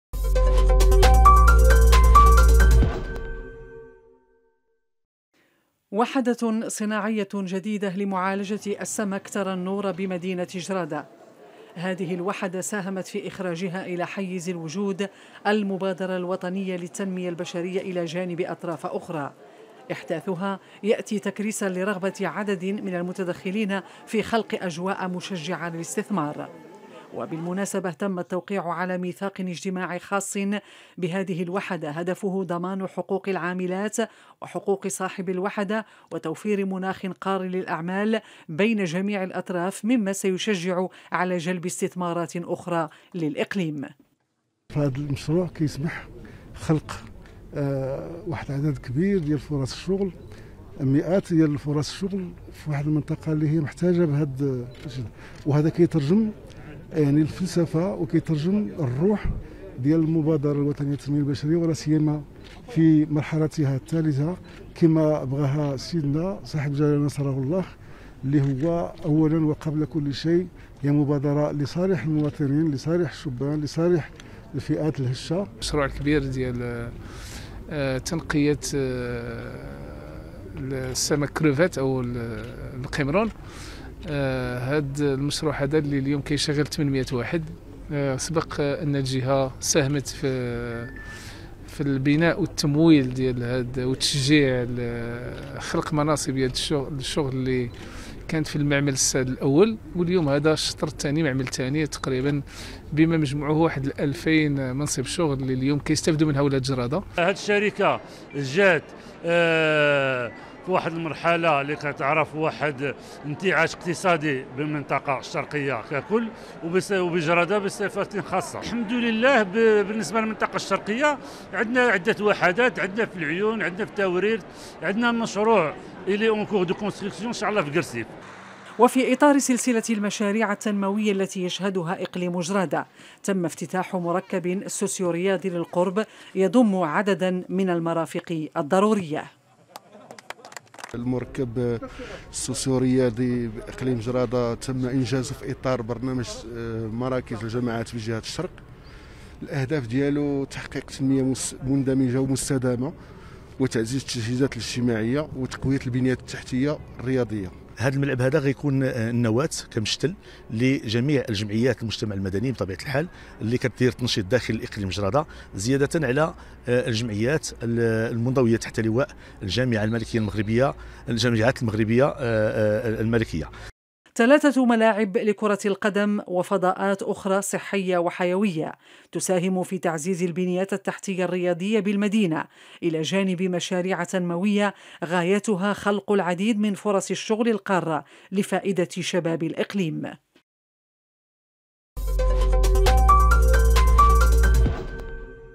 جرادة .. وحدة لمعالجة السمك ومركب رياضي
جرى الخميس 11 نونبر، تدشين وحدة صناعية لمعالجة السمك (تنقية القمرون)، والتي ستساهم في توفير حوالي 1000 منصب شغل قار من اليد العاملة المحلية.
تم إنجاز هذه الوحدة، التي تم تدشينها بمناسبة تخليد الذكرى الـ 46 للمسيرة الخضراء، وفي إطار الجهود المبذولة لتشجيع الاستثمار، وخلق فرص الشغل بالإقليم، بتمويل مشترك بين المبادرة الوطنية للتنمية البشرية (6 ملايين درهم)، ومجلس جهة الشرق (6 ملايين درهم)، والمركز الجهوي للاستثمار لجهة الشرق (1.3 مليون درهم)، وشركة "MMSS" صاحبة المشروع (8 ملايين درهم)، ثم شركة العمران (صاحب المشروع المنتدب).
وبهذه المناسبة، تم توقيع ميثاق اجتماعي خاص بهذه الوحدة بين عمالة إقليم جرادة، والمديرية الجهوية لوزارة الادماج الاقتصادي والمقاولات الصغرى والشغل والكفاءات (قطاع الشغل)، والشركة المشغلة (MMSS)، وممثلو المنظمات النقابية بإقليم جرادة.
يرجى الاشتراك..